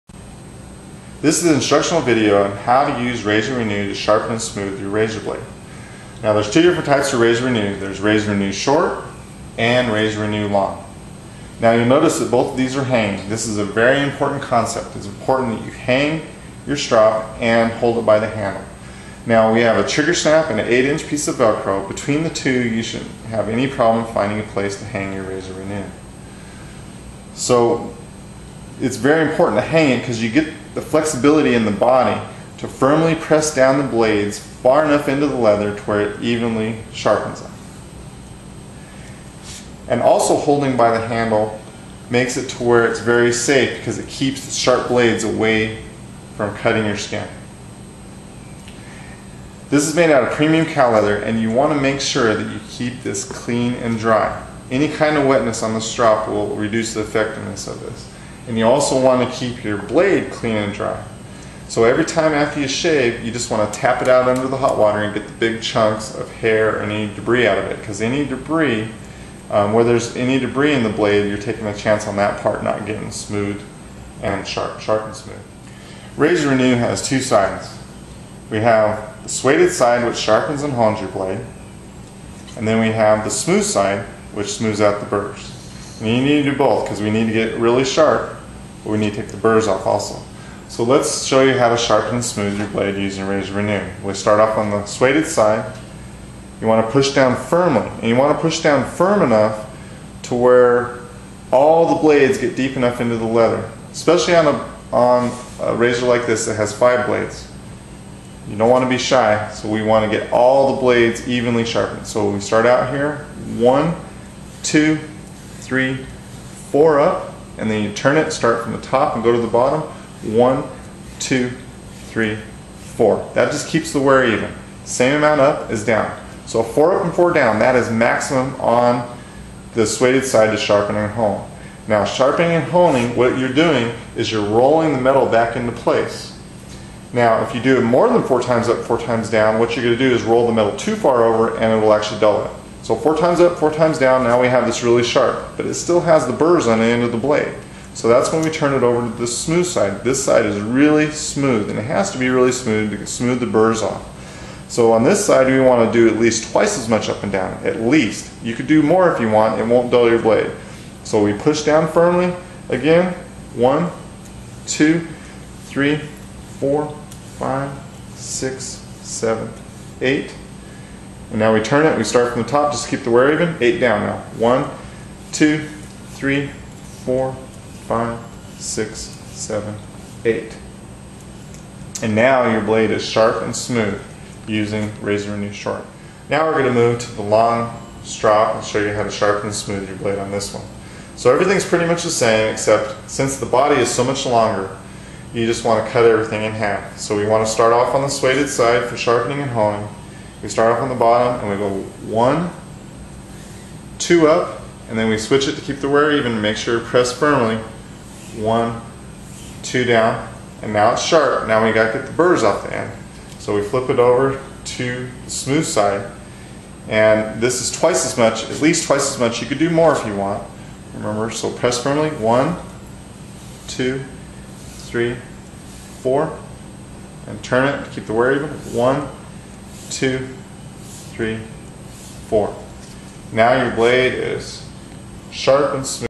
Razor Renew Short & Long Razor Blade Instructional Video
Razor Renew Short and Long Manually Sharpens and Smoothes Razor Blades, Knives, Scissors and Other Fine Cutting Instruments!